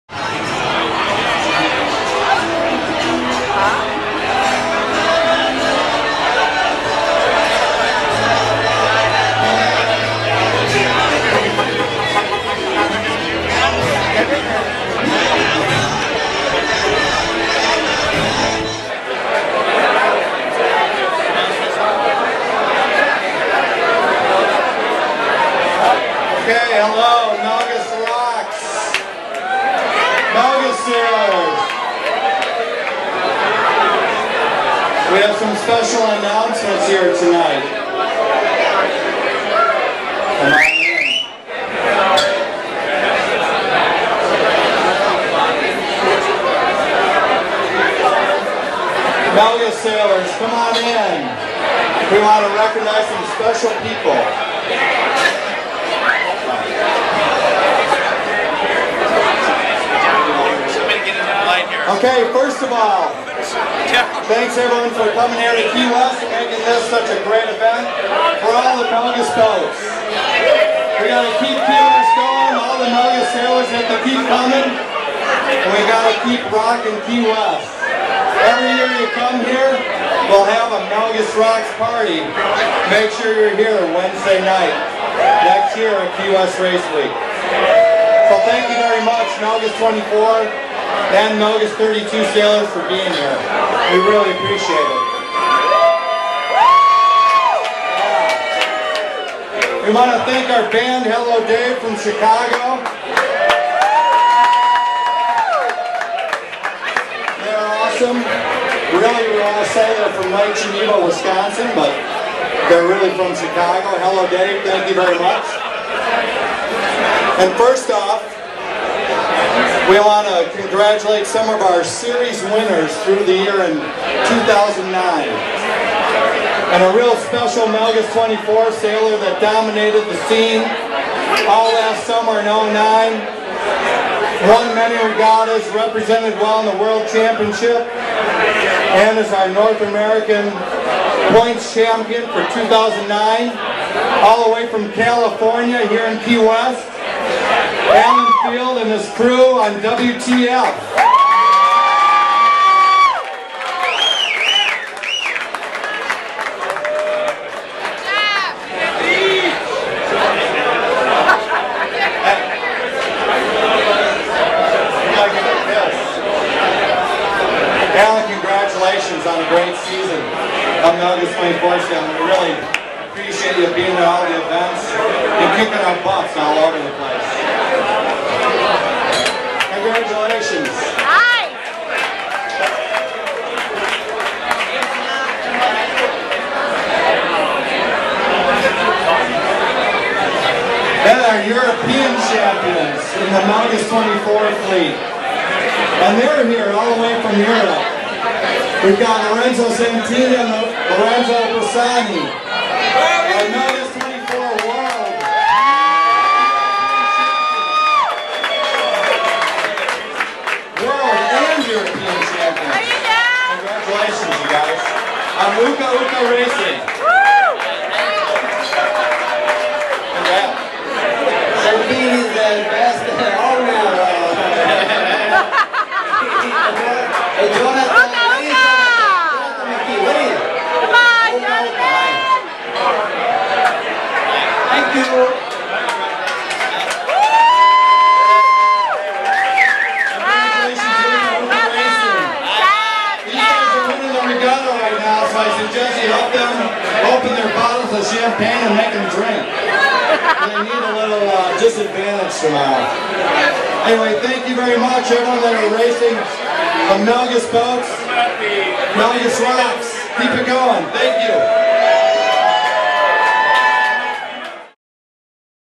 Melges Rocks Key West Race Week 2010
Sponsored and supported by Melges Performance Sailboats, Melges Rocks Key West with a grand party for Melges 24 and Melges 32 sailors. Includes International Melges 24 Class North American Ranking Champion Alan Field as well as World and European Champion Lorenzo Bressani and Uka Uka owner Lorenze Santini